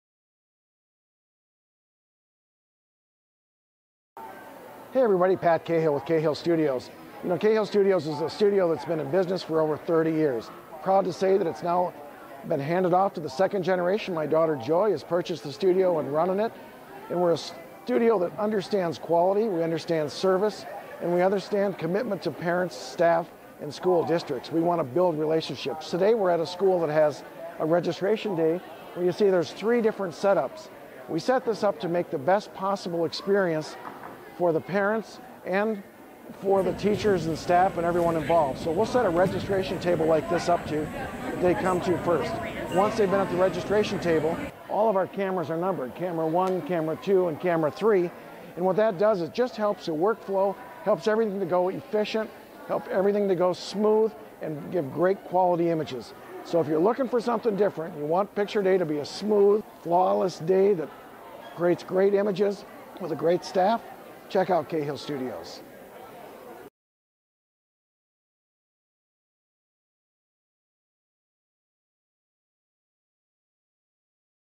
Cahill Studios of Photography School Pictures Promo 2018
Today’s tip is no matter how busy you are always take time to promote your business. This is a short video we made at a school pre-registration day to promote Cahill Studios of Photography. Joy Cahill and her team have set up an efficient 3 camera shoot that allows the Cahill team to have a stress free day, where the team can focus on creating a great customer experience and cherished school pictures.